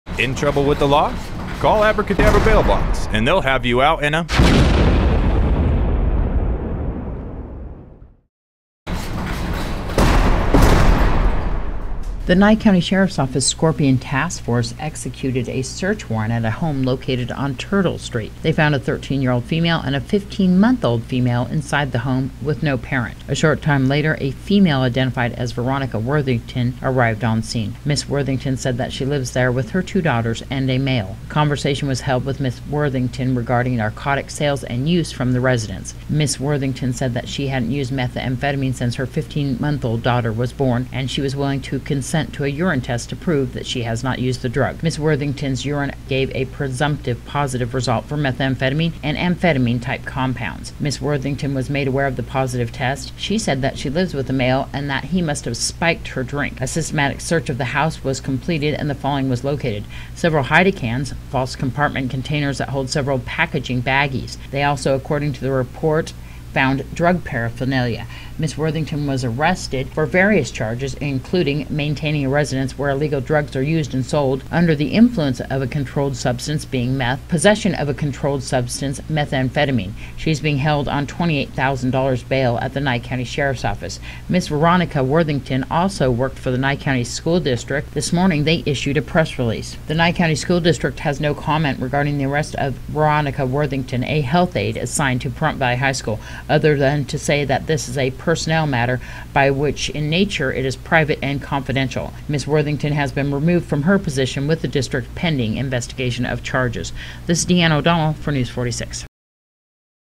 01/31/2013 Nye County Sheriff Office Arrest Verona Worthington
The Pahrump Valley High School health aid was arrested yesterday for various charges related to the sale and use of methamphetamine.